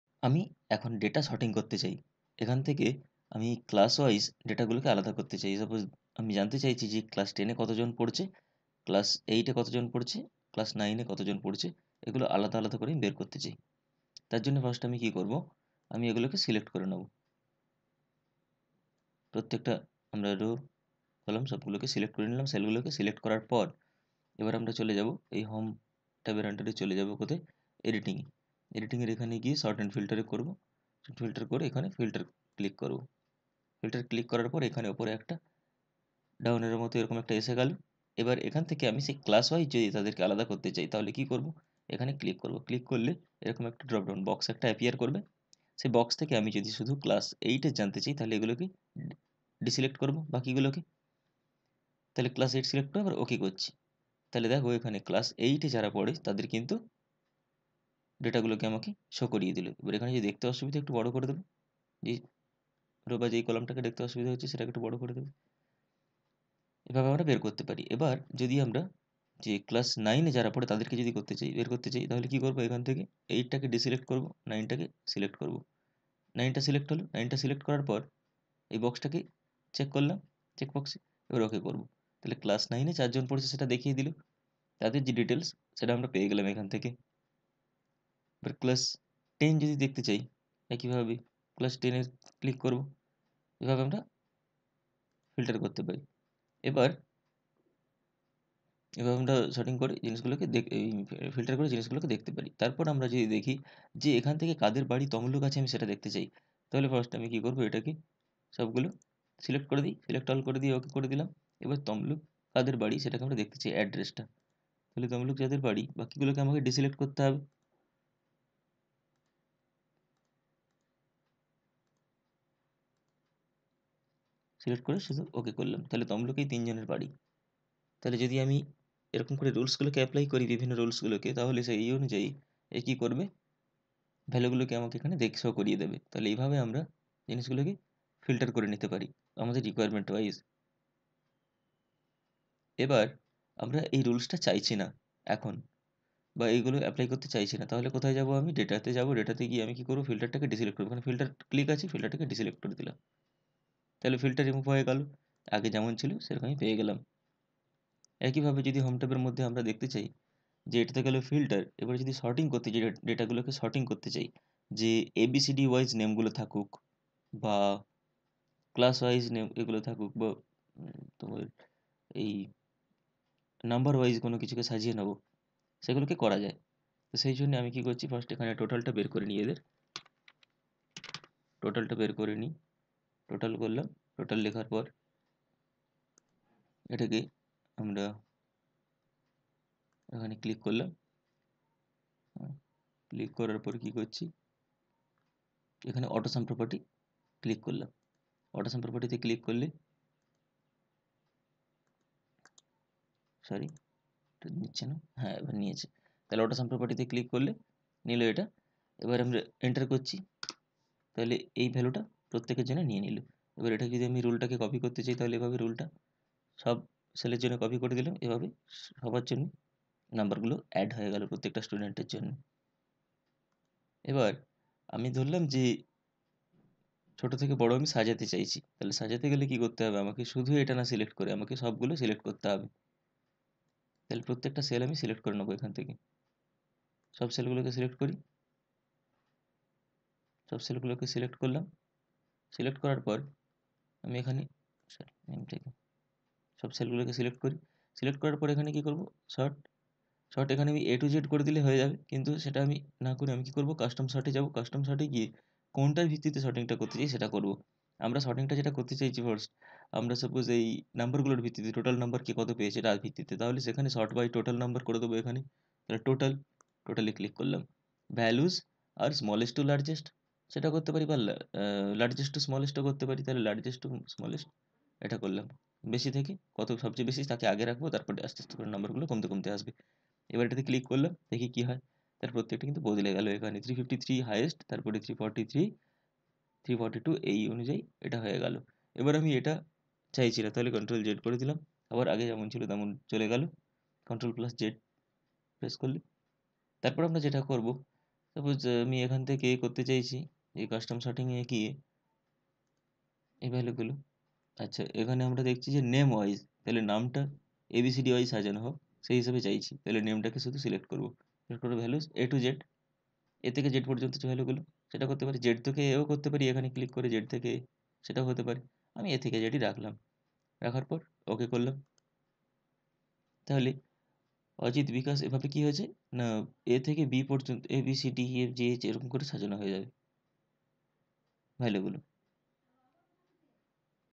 Data Filtering and Sorting in Excel Lesson- 11 | সর্টিং এবং ফিল্টারিং পাঠ - ১১
কিভাবে ডেটা ছোট থেকে বড় বা বড় থেকে ছোট সাজাবেন একটি টেবিল এ এবং কিভাবে একটি কমন বৈশিষ্ট্য অনুযায়ী কিছু ডেটা দেখবেন শেখানো হয়ছে। আশাকরি ভিডিওটি কাজে আসবে। সবার সাফল্য কামনা করি।
This is about how to sort data and filter specific set of data from a table.
It is a very important option in MS excel that help us to know and group data according to some property. Sometimes it is necessary to organize the data in ascending and descending order.
Hope this will help you to learn MS excel. Wish you a great success.